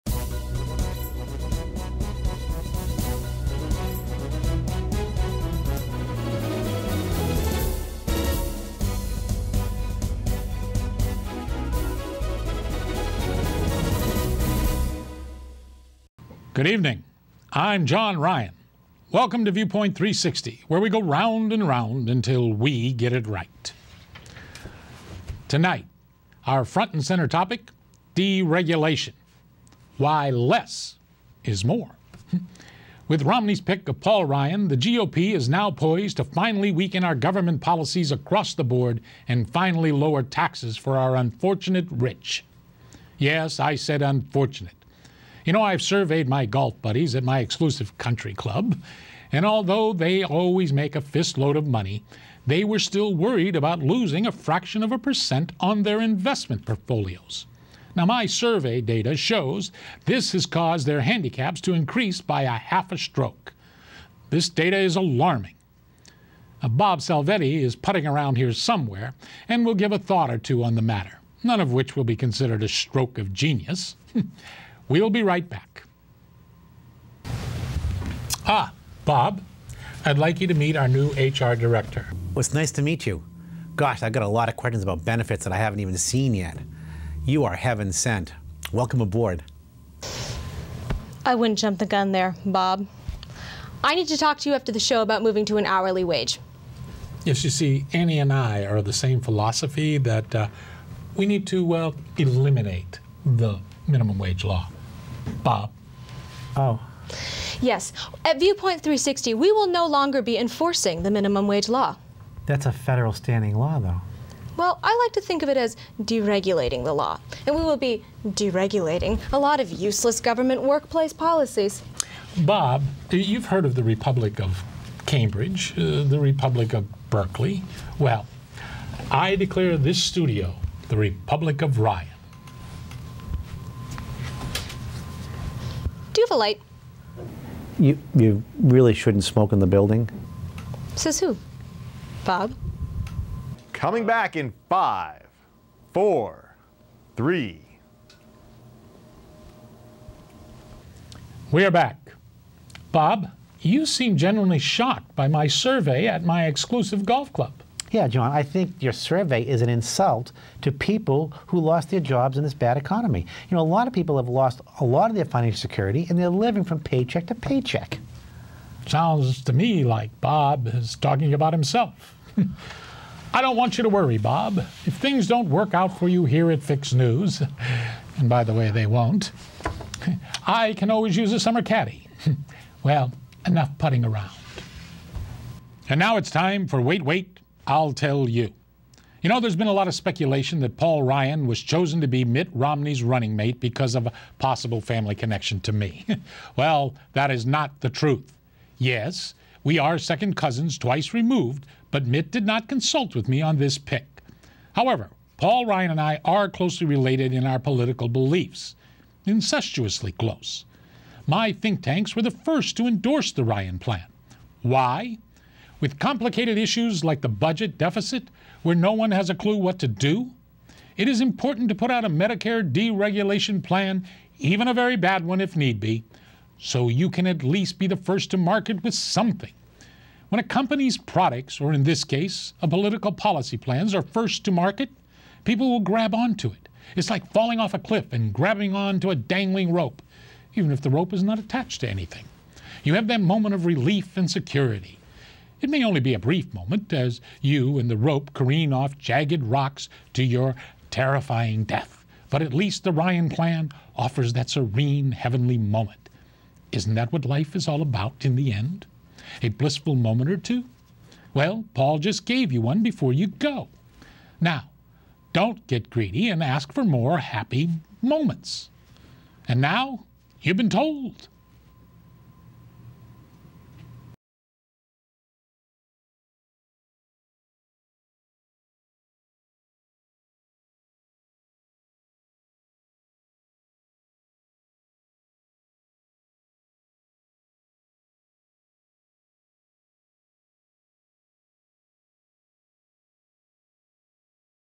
Episode 3 Deregulating HR Policy
Fox parody webisode series discusses current events and politicals.  Series also goes behind the scenes and looks at the personal relationships at the news show.

John Depew as John Ryan
Bob Gigliotti as Bob Salvetti
Anna Rizzo as Annie The HR Director
Scott Berger as Tim The Janitor
Directed by Bob Gigliotti
Written by Bob Gigliotti
Producer Steve Madore
Camera Work by Kim Allyn
Musical Composed by Mark Styles
Graphic Design by Zach Stone
Executive Producer Bob Gigliotti in Association with BevCam
Created by Bob Gigliotti